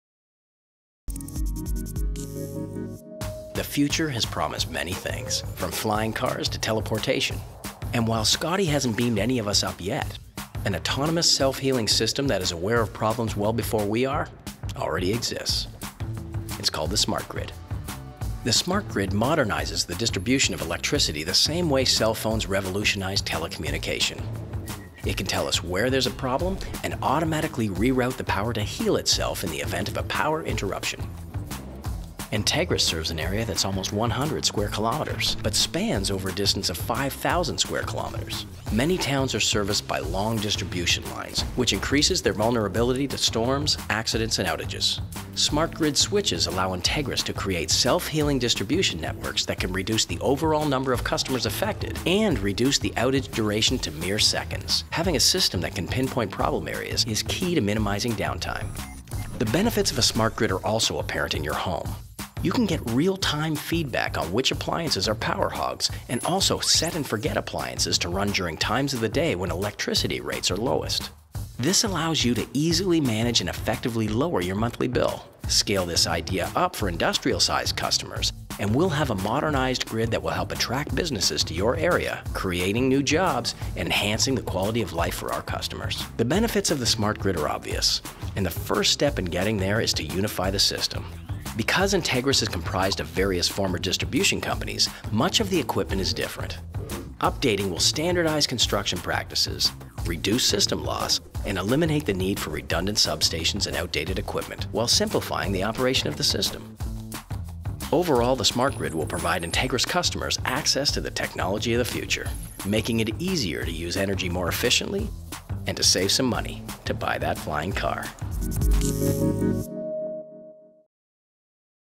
Smart Grid
What is the smart grid? How will it benefit Entegrus customers? This video explains both.